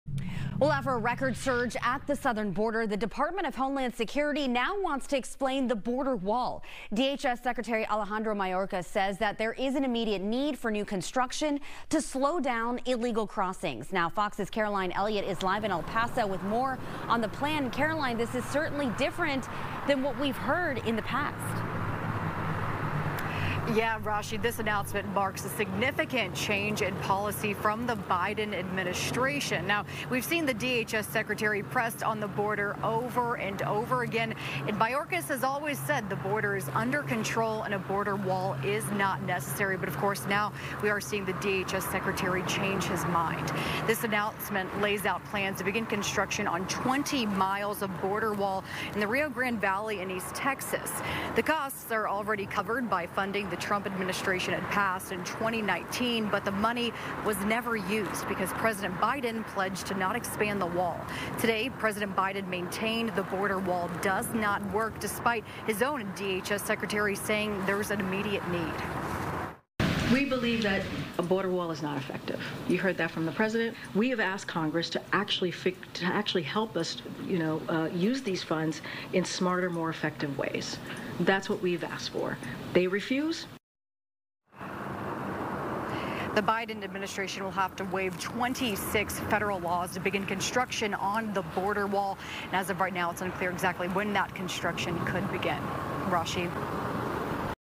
Homeland Security's plan to expand border wall
Fox's Caroline Elliott is live in El Paso with more details on the plan.